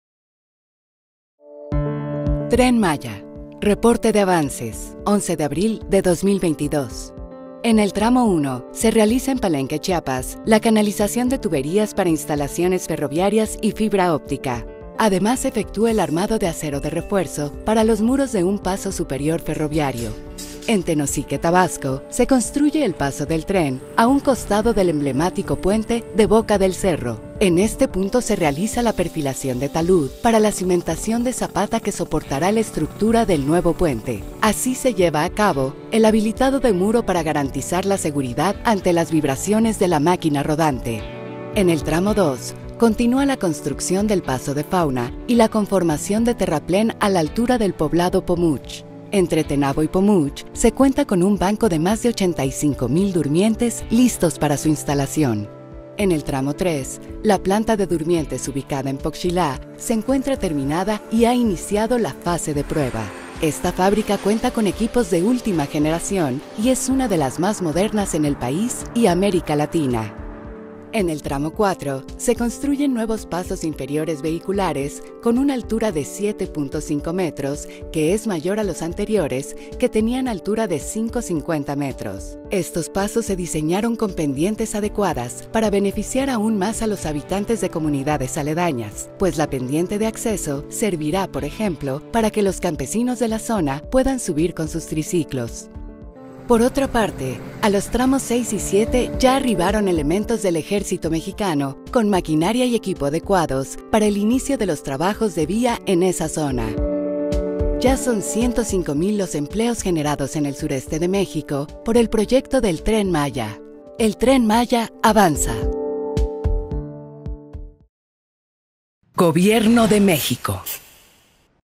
Reporte de Avances de Obra. Tren Maya 11/04/22
Ciudad de México, 11 de abril de 2022.

En el avance de sus mil 500 kilómetros de ruta, el Tren Maya ha generado 105 mil empleos en el sureste de México y en las últimas semanas se reforzó con la llegada de elementos del Ejército Mexicano para el inicio de los trabajos en los tramos 6 y 7.

En Palenque, Chiapas, correspondiente al Tramo 1, se realiza la canalización de tuberías para instalaciones ferroviarias y fibra óptica. Además, se efectúa el armado de acero de refuerzo para los muros de un paso superior ferroviario, mientras en Tenosique, Tabasco, se construye el paso del tren a un costado del emblemático puente de Boca del Cerro.

En este punto, se realiza la perfilación de talud para la cimentación de zapata que soportará la estructura del nuevo puente. Así se lleva a cabo el habilitado de muro para garantizar la seguridad ante las vibraciones de la máquina rodante.

En el Tramo 2, Escárcega-Calkiní, continúa la construcción de un paso de fauna más y la conformación de terraplén a la altura del poblado Pomuch. Entre Tenabo y Pomuch, asimismo, se cuenta con un banco de más de 85 mil durmientes listos para su instalación.

En el Tramo 3, Calkiní-Izamal, quedó terminada la planta de durmientes ubicada en Poxilá, y ya comenzó la fase de prueba. Esta fábrica cuenta con equipos de última generación y es una de las más modernas de México y de América Latina.

En el Tramo 4, Izamal-Cancún, se construyen nuevos pasos inferiores vehiculares con altura de 7.50 metros, que es mayor a la altura de los anteriores, que era de 5.50 metros.

Estos pasos se diseñaron con pendientes adecuadas para beneficiar aún más a los habitantes de comunidades aledañas, pues la de acceso servirá, por ejemplo, para que los campesinos de la zona puedan subir con sus triciclos.

El Tren Maya avanza y es desarrollo para el país y justicia social para el sureste.